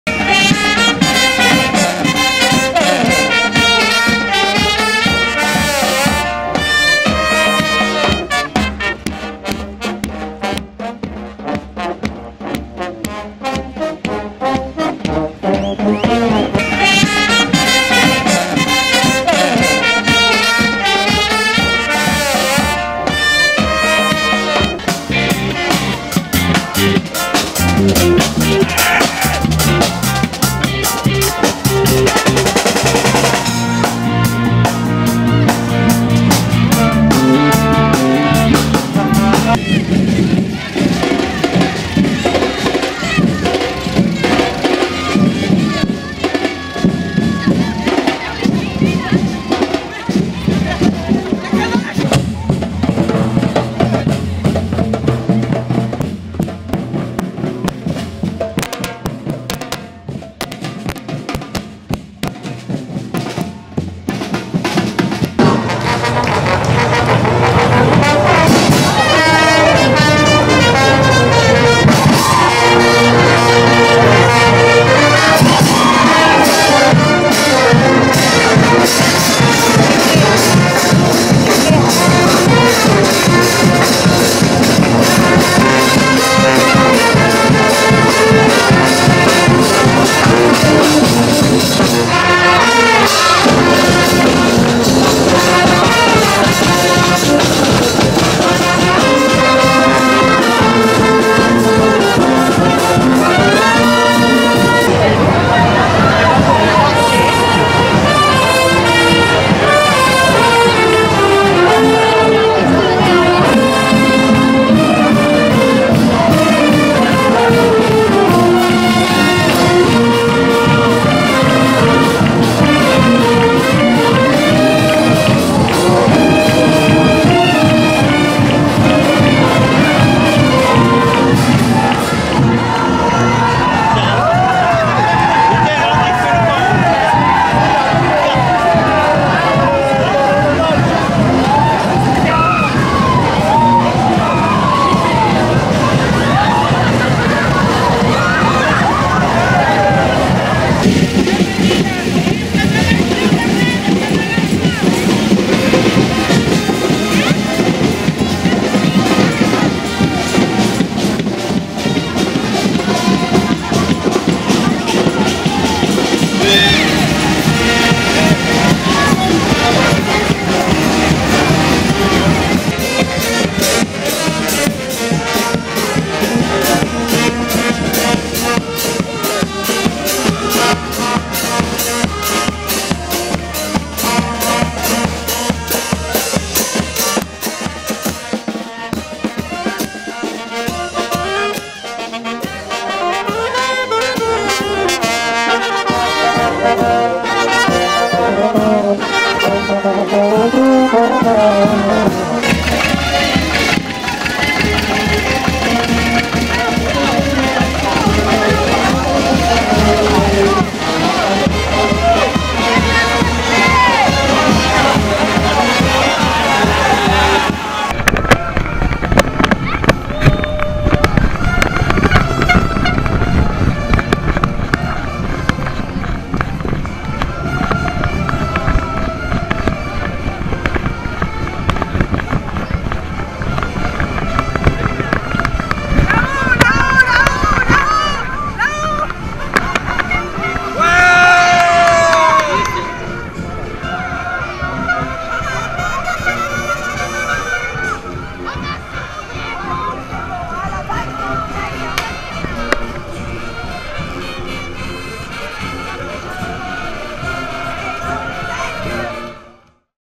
Cavalcade du samedi
A l'instar de la cérémonie d'ouverture des Jeux olympiques, la cavalcade du samedi a eu lieu, en partie, sous la pluie.